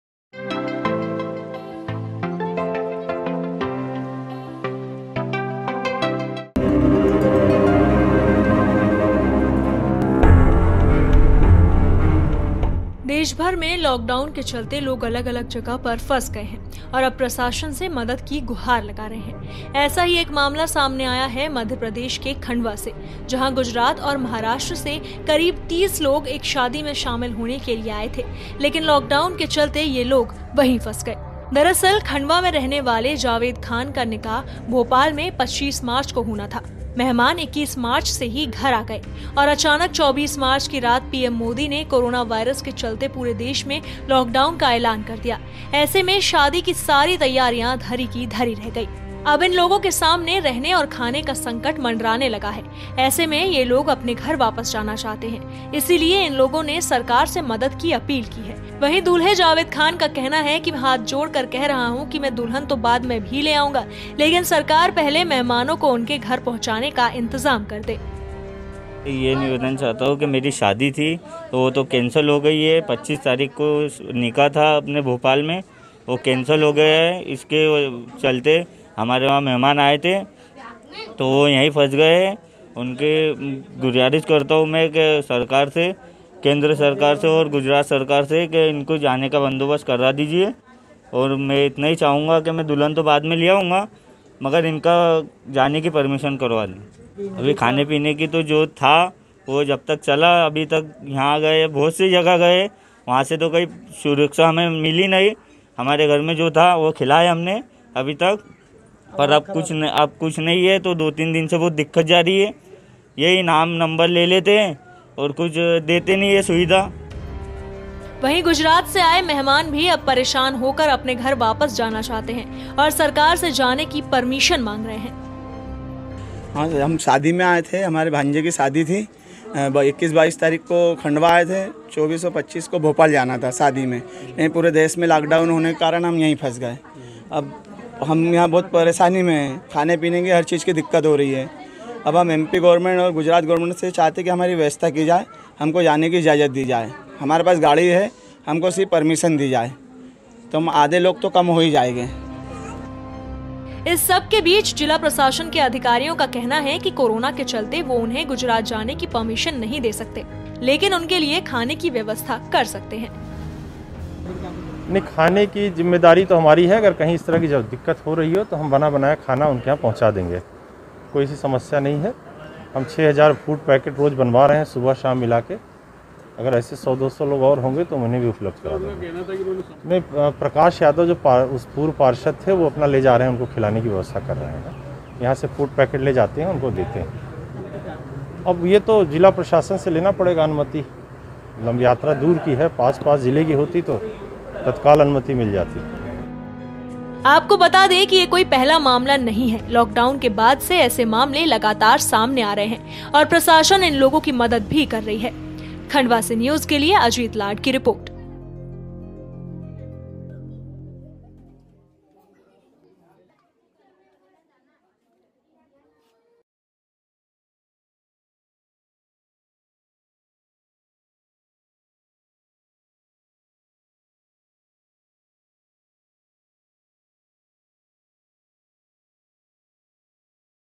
Khandwa में फंसे शादी में शामिल होने आए मेहमान, प्रशासन से मदद की गुहार
देशभर में लॉक डाउन के चलते लोग जगह - जगह पर फंस गए हैं और अब प्रशासन से मदद की गुहार लगा रहे हैं... ऐसा ही एक मामला सामने आया है मध्यप्रदेश के खंडवा से. जहां गुजरात और महाराष्ट्र से करीब 30 लोग एक शादी में शामिल होने के लिए आए थे लेकिन लॉकडाउन के चलते ये लोग यहीं फंस गए...